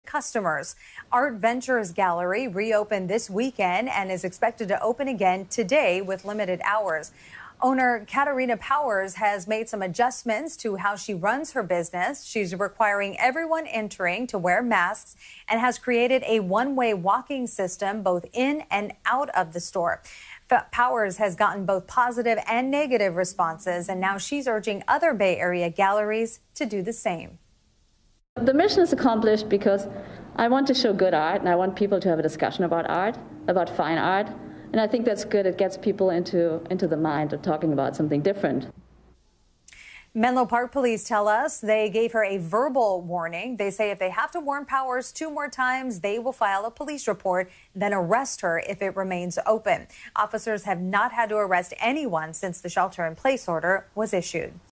Coronavirus Update: Menlo Park Art Gallery Defies Shelter-In-Place Order
An art gallery in San Mateo County is defying the shelter-in-place order due to the coronavirus pandemic, reopening its doors to customers. (5/6/20)